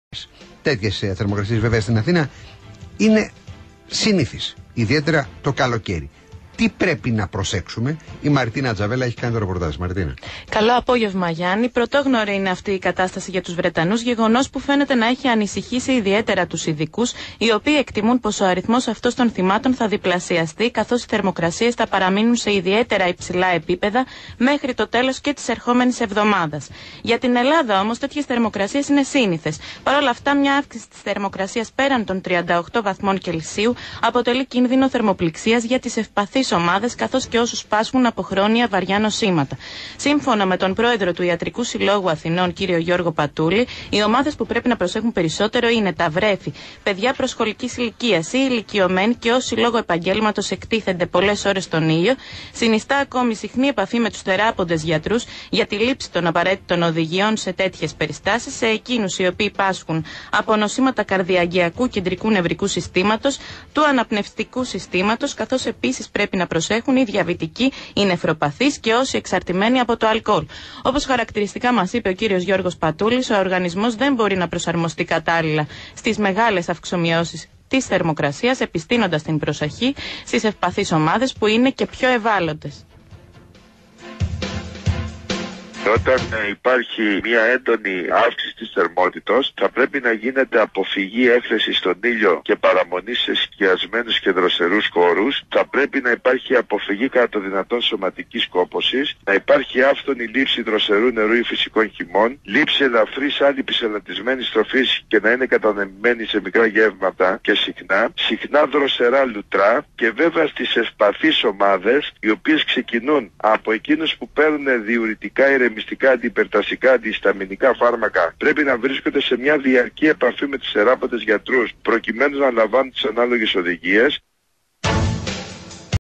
O Γ.Πατούλης στο Βήμα 99,5 fm για την προστασία απο την θερμοπληξία.19-7-2013
Σημεία συνέντευξης Δημάρχου Αμαρουσίου και Προέδρου Ι.Σ.Α. κ. Γιώργου Πατούλη στο Βήμα 99,5 fm για την προστασία απο την θερμοπληξία.19-7-2013